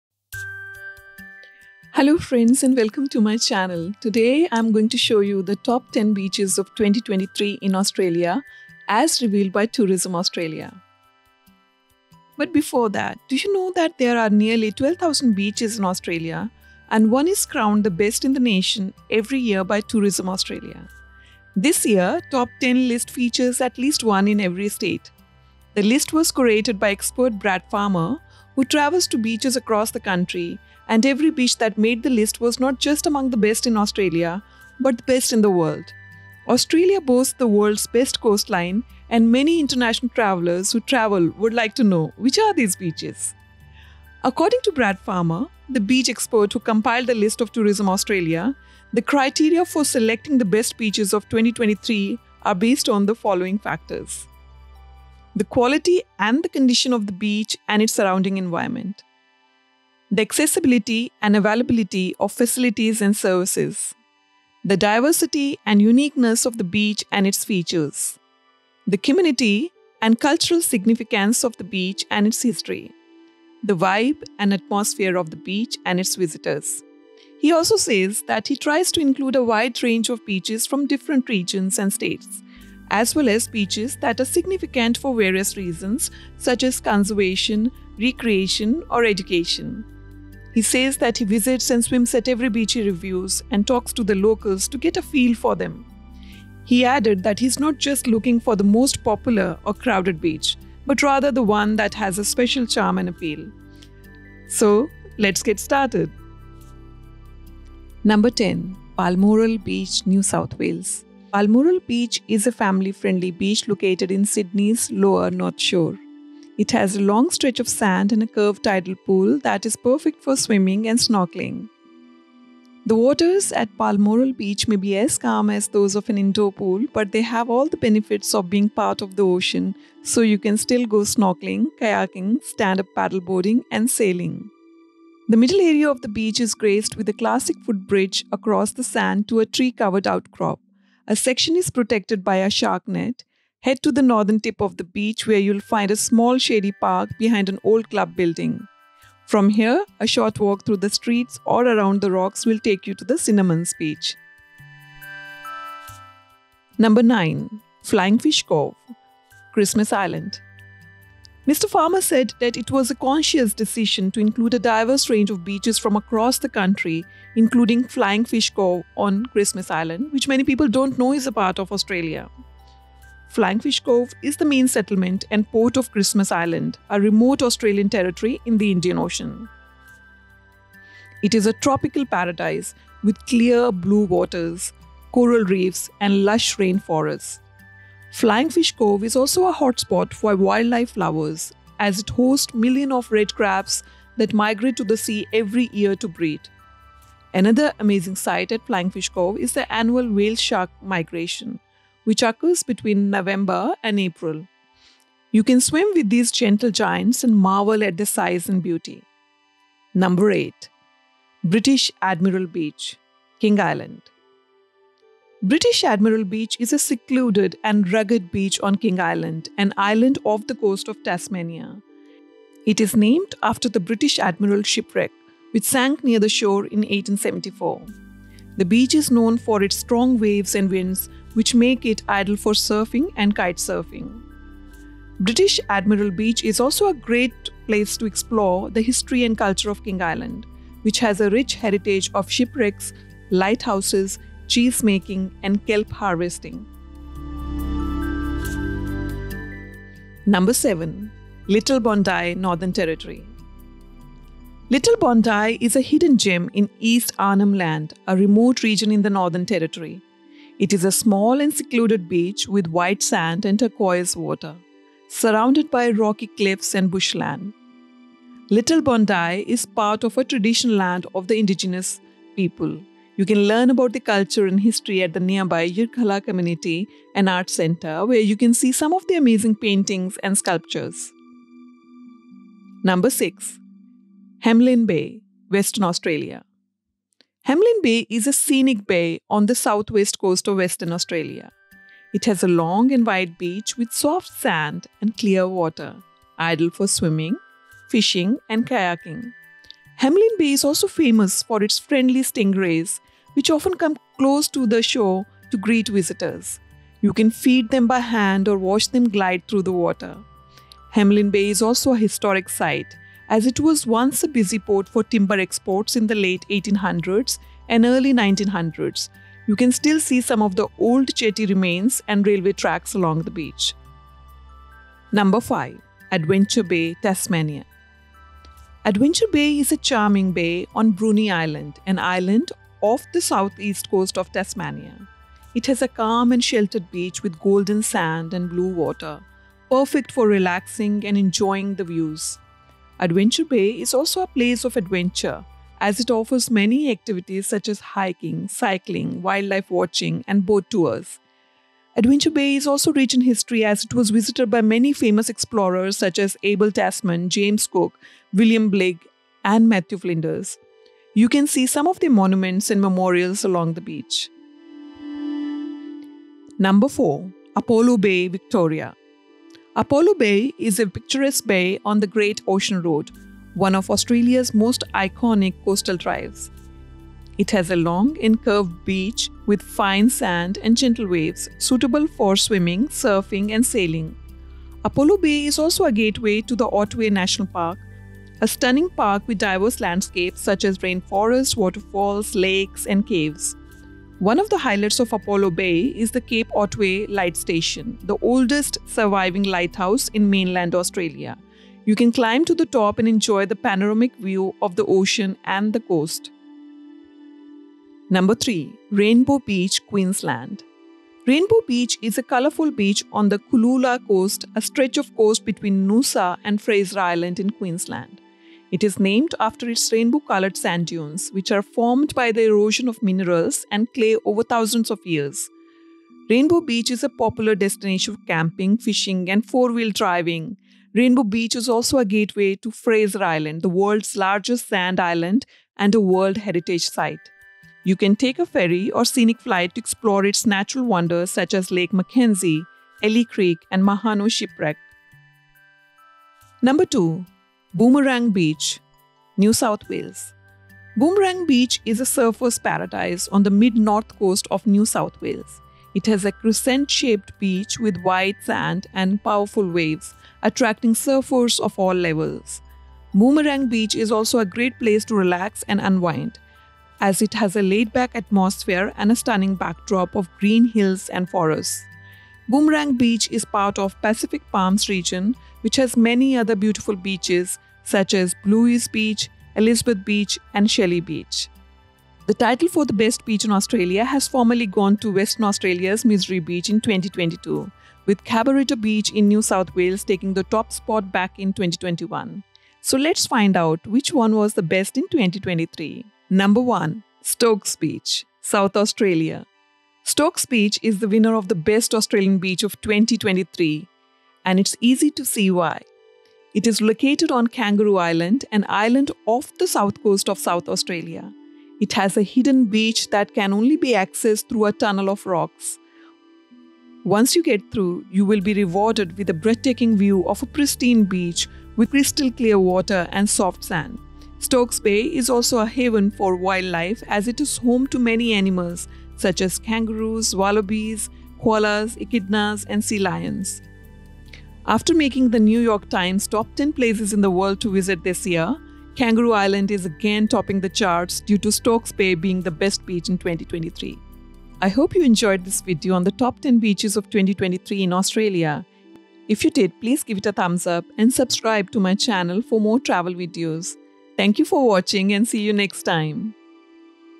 Tourism Australia's Top 10 Australian Beaches of 2023
We're spoilt for choice when it comes to beaches and with summer almost here, Tourism Australia has announced its top ten beaches in Australia for 2023 :

The top ten beaches in Australia for 2023:
1. Stokes Bay, Kangaroo Island, South Australia
2. Boomerang Beach, Mid North Coast, New South Wales
3. Rainbow Beach, Cooloola Coast, Queensland
4. Apollo Bay, Great Ocean Road, Victoria
5. Adventure Bay, Bruny Island, Tasmania
6. Hamelin Bay, South-West, Western Australia
7. Little Bondi Beach, East Arnhem Land, Northern Territory
8. British Admiralty Beach, King Island, Bass Strait, Tasmania
9. Flying Fish Cove, Christmas Island
10. Balmoral Beach, Sydney Harbour, NSW
Australia, beach, travel, tourism, destination, coast, ocean, sea, sand, sun, surf, nature, wildlife, adventure, relaxation, culture, history, beauty